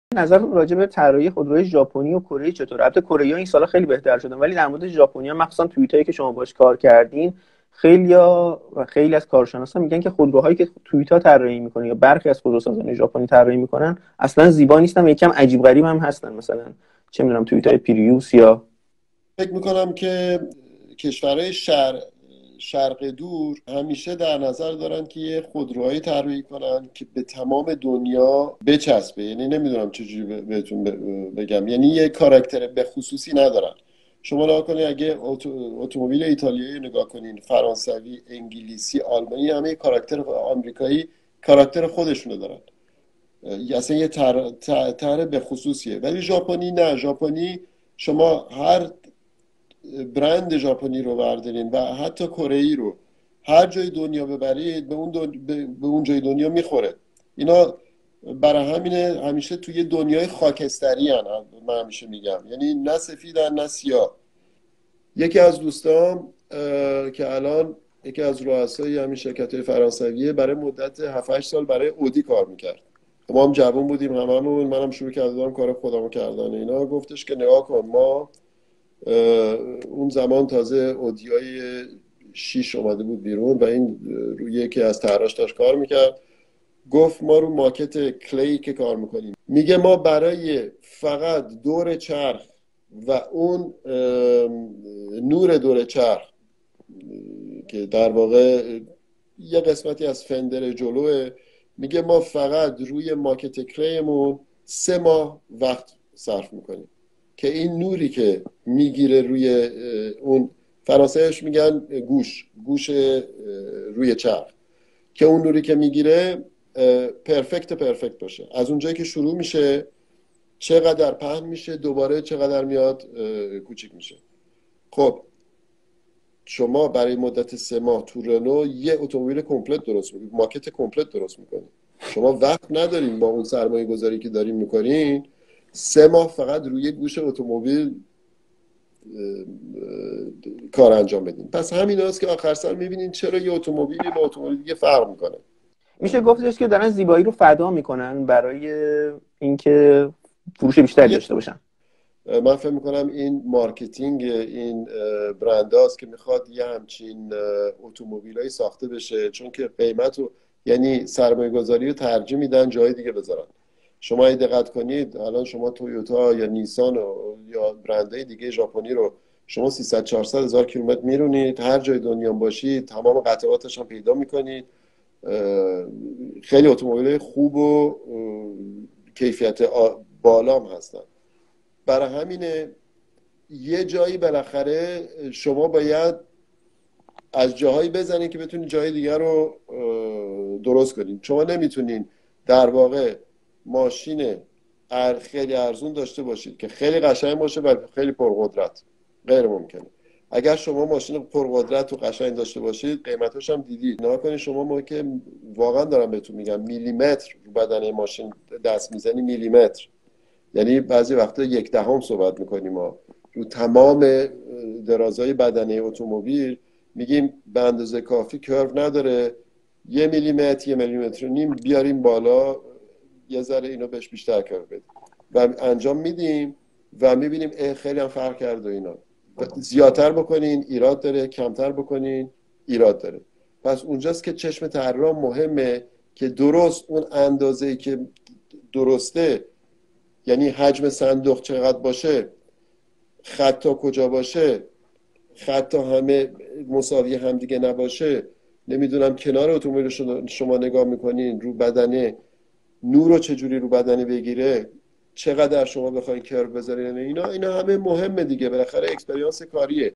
چرا خودرو ژاپنی زشت هستند؟
هرمیداس اتابکی طراح بزرگ ایرانی درباره طراحی خودرو ژاپنی توضیح می دهد
شما چه نظری دارید